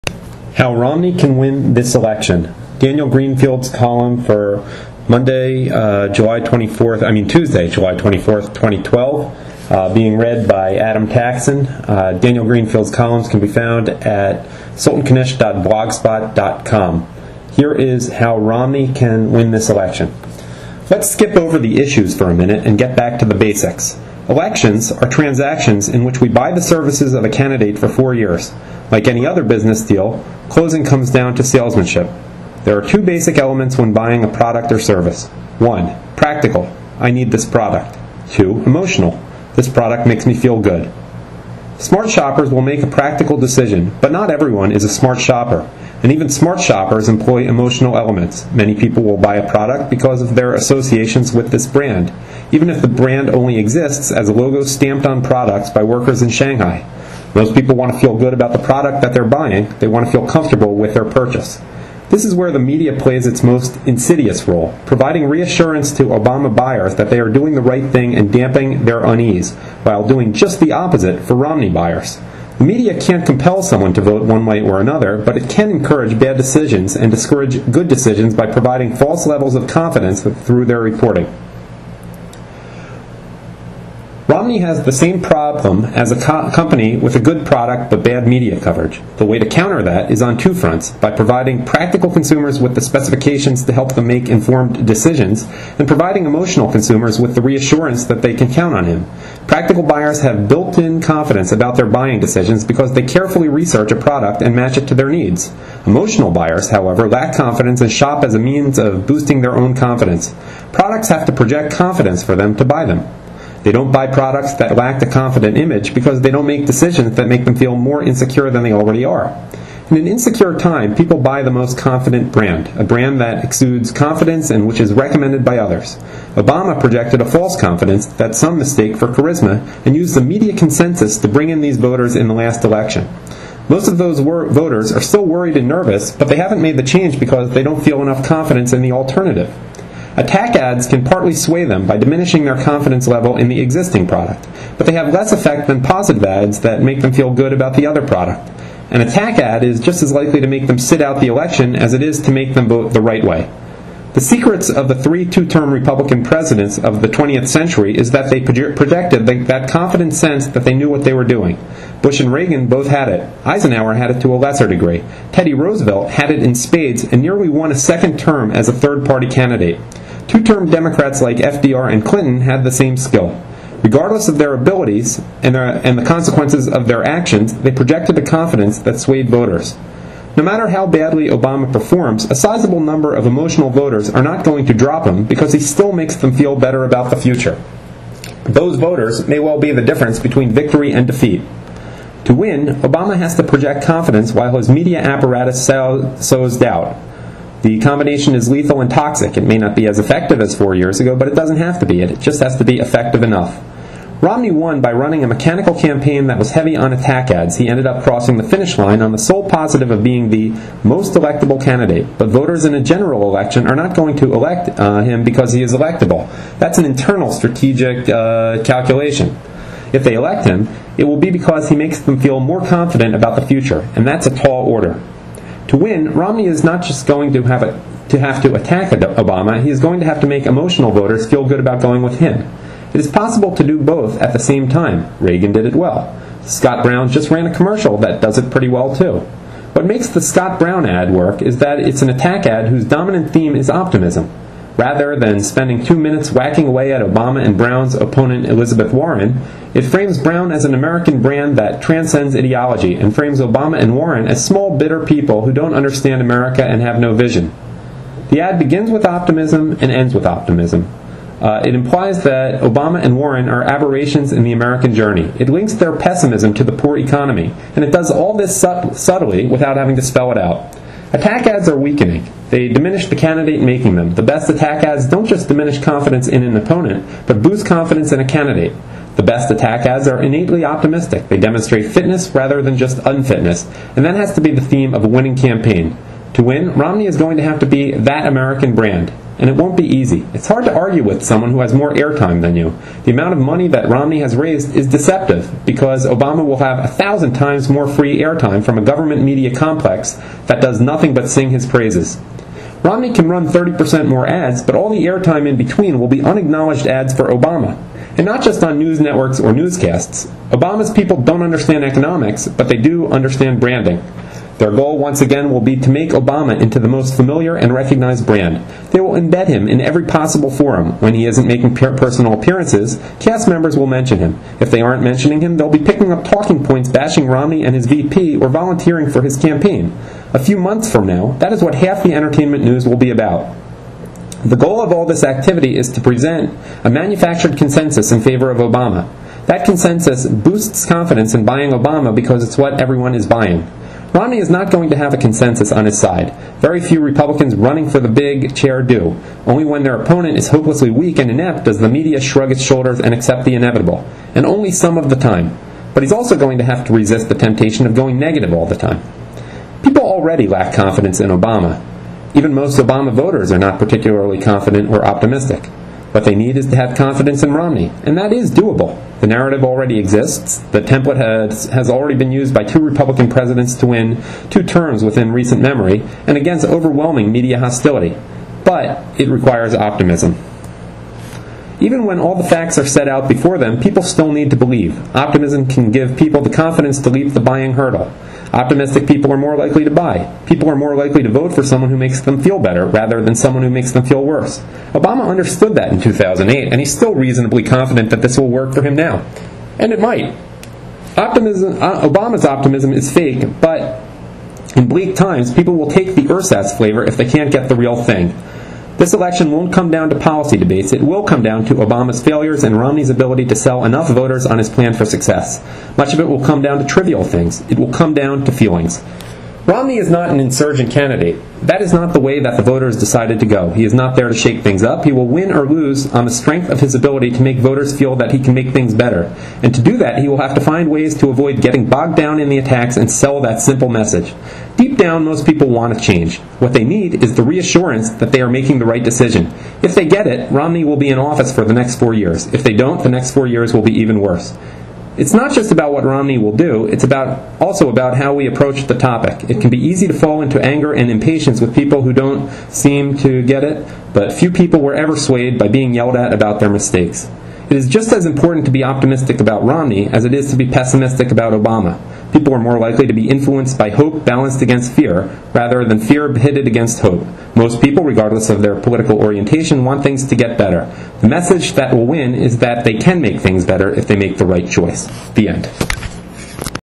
AUDIO VERSION: "How Romney Can Win This Election" (Daniel Greenfield 7/24/12 column)
Daniel Greenfield's columns can be found at sultanknish.blogspot.com.

Read by Adam Taxin with the full permission of the author.